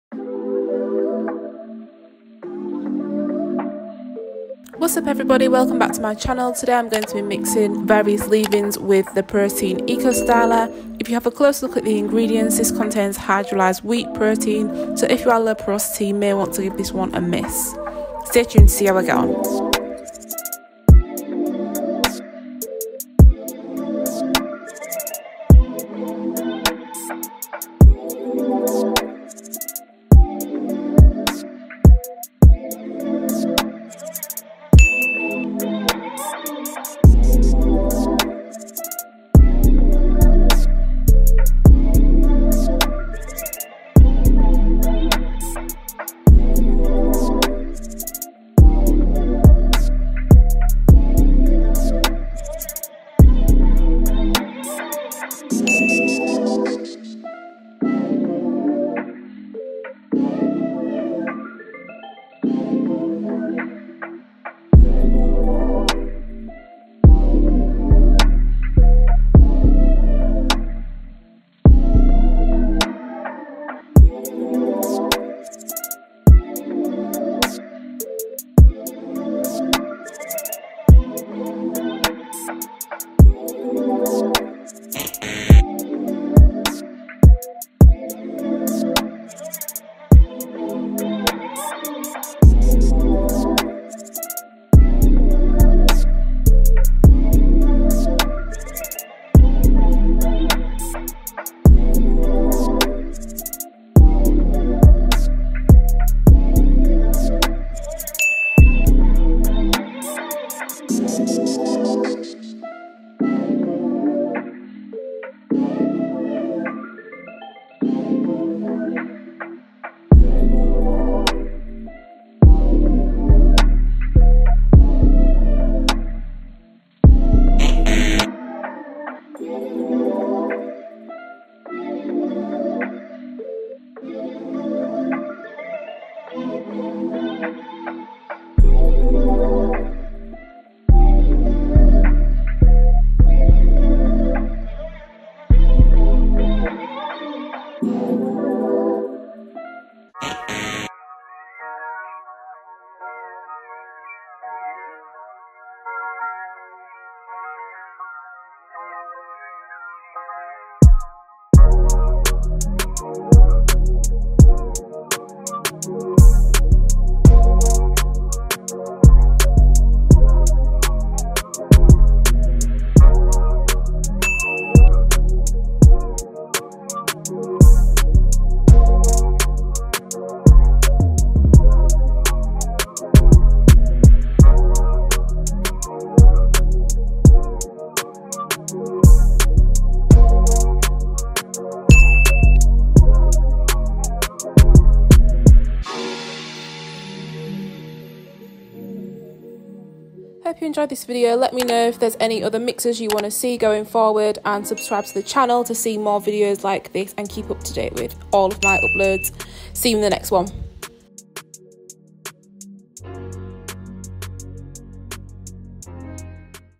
HOW TO AVOID GEL FLAKING? | MIXING LEAVE INS WITH PROTEIN ECO| Kekekurly
n this video I experiment with mixing various leave in creams/conditioners with Eco Styler Protein Gel. The purpose of this is so you can see what will mix well when applied to your hair, and what causes the white build up when the products don't mix well. The idea of mixing a leave in with a styling gel is so that you can get both moisture and definition from your style.